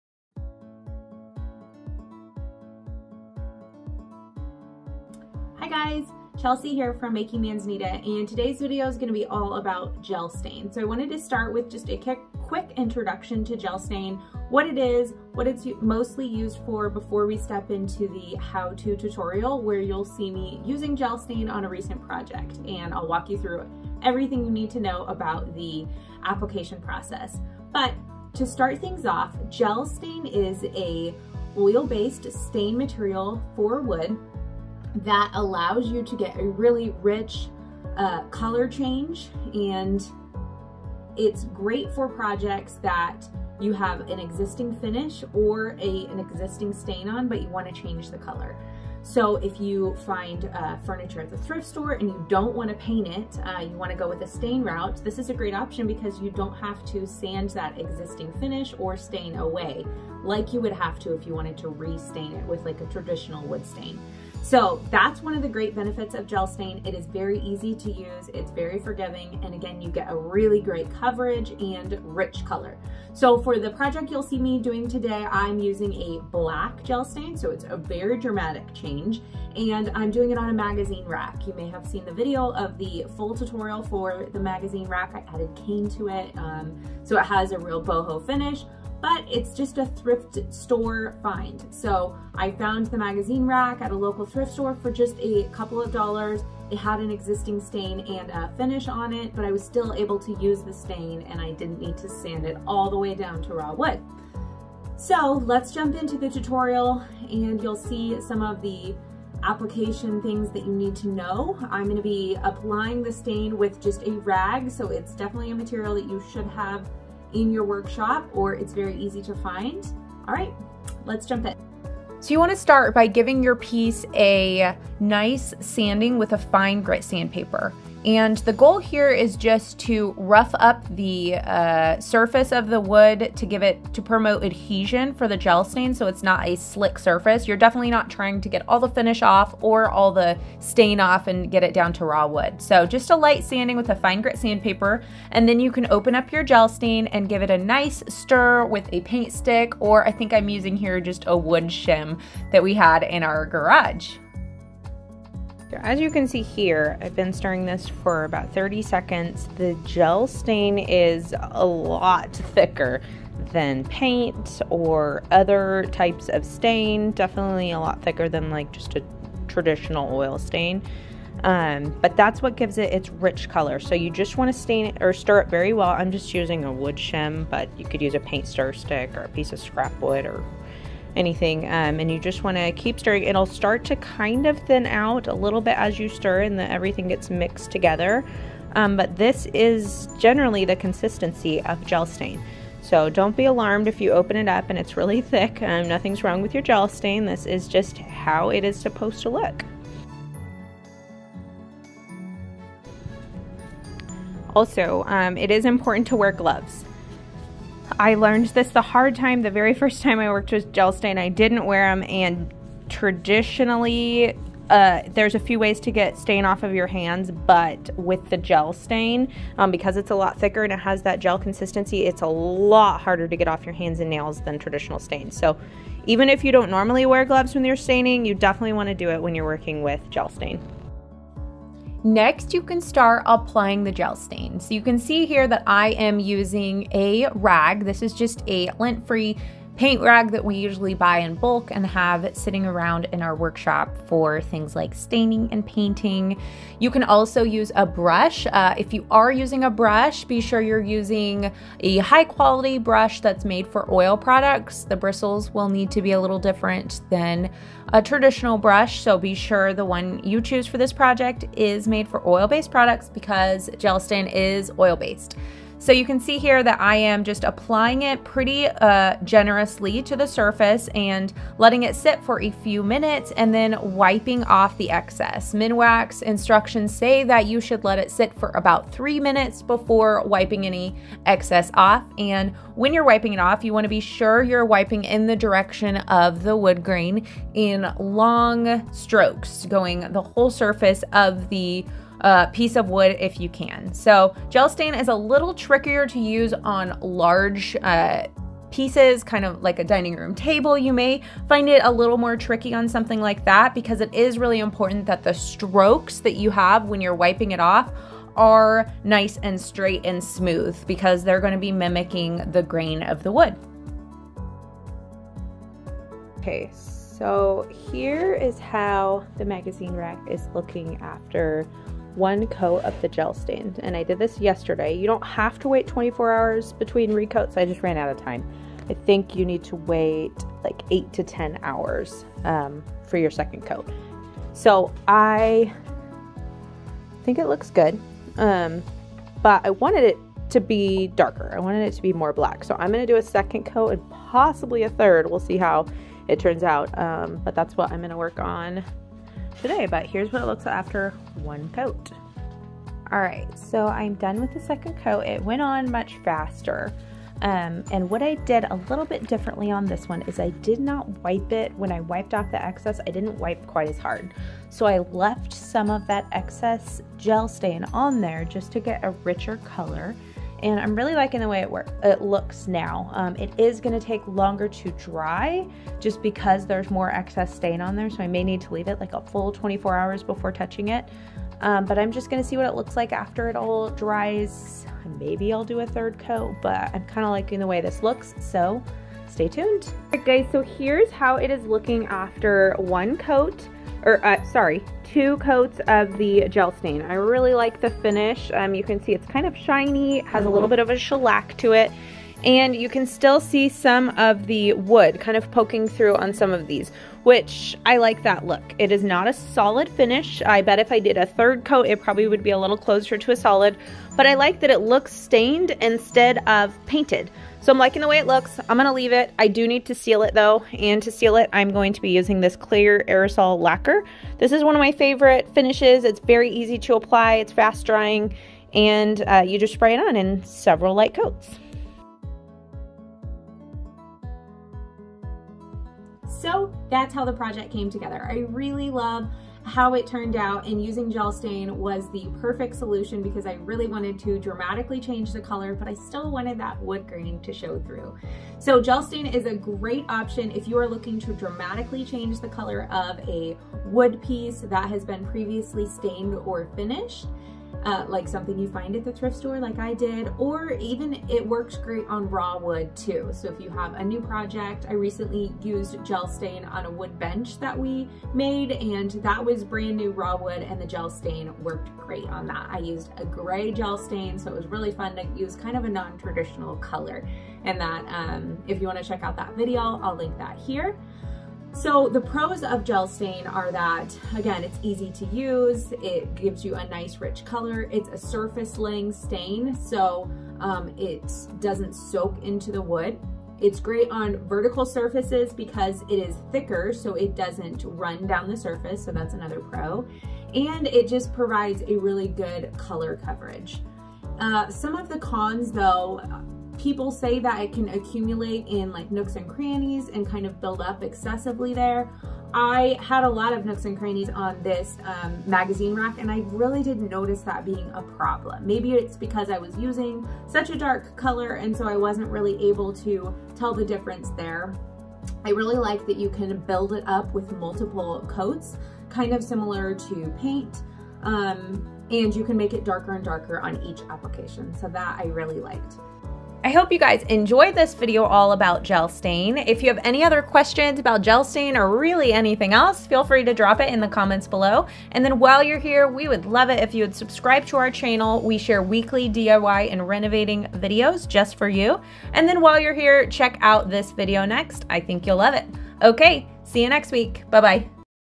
How To Apply Gel Stain With A Rag
Are you curious about how to use Minwax gel stain over finished thrift store furniture? Even if your wood has been stained and has an existing finish, you can apply gel wax vs regular stain to refresh it and give it a brand new look.

You won't believe this before and after using Minwax black gel stain on this magazine rack. Follow along with our step-by-step tutorial on how to put on gel stain with a rag and use our application tips and tricks.

- Black gel stain
- 220-grit sandpaper
- Lint-free paint rags
- Gloves
- Paint stir stick
- Clear lacquer finish

Video Summary:
0:00 Introduction
2:02 Sand
3:52 Apply gel stain
6:57 Seal
7:16 Final thoughts